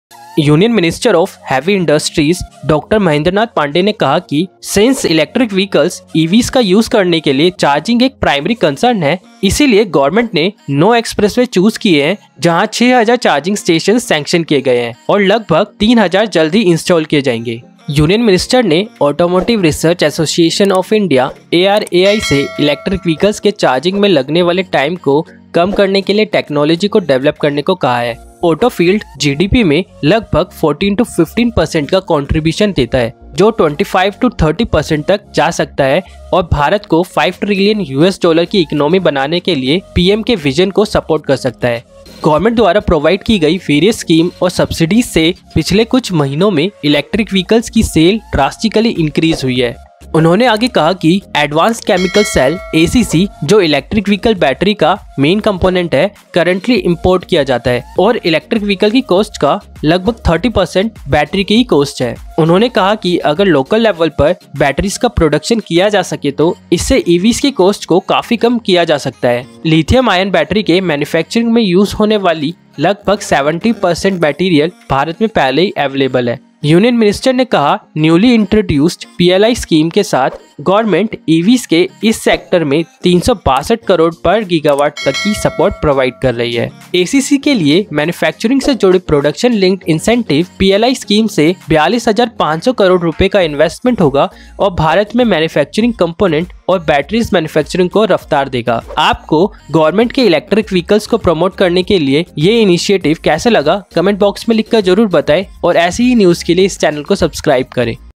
Govt Plans To Install 6,000 EV Charging Stations Along Nine Expressways
Union Minister Dr Mahendra Nath Pandey said that auto field’s contribution to GDP can go up to 25-30 per cent and can support making India a USD 5 trillion economy.